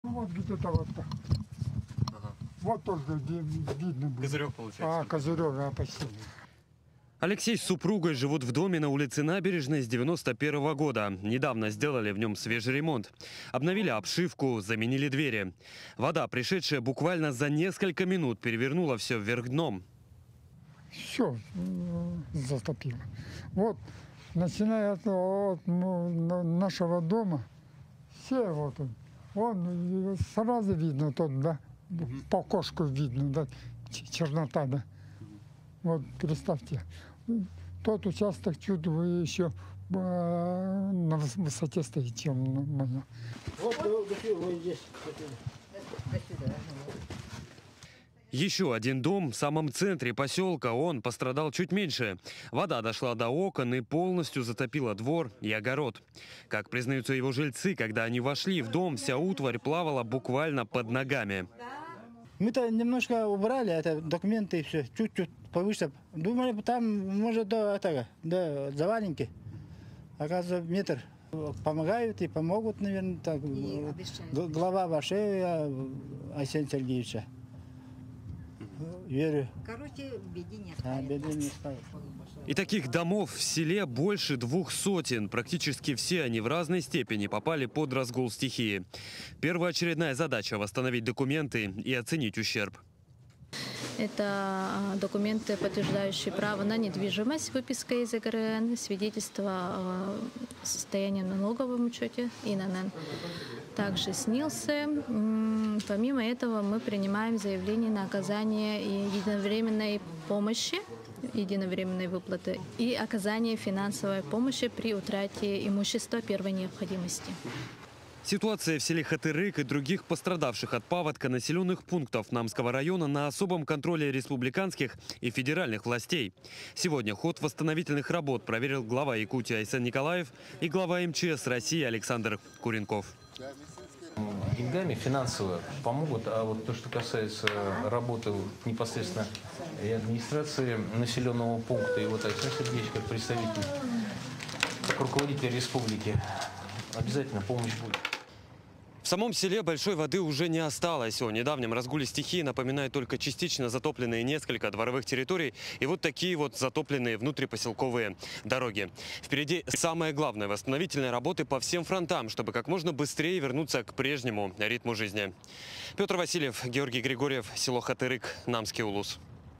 Глава МЧС России проверил ход восстановления села Хатырык в Якутии
Населенные пункты Намского района постепенно освобождаются от воды,  активно идут работы по восстановлению социальных, транспортных и энергетических объектов, оказывается адресная помощь.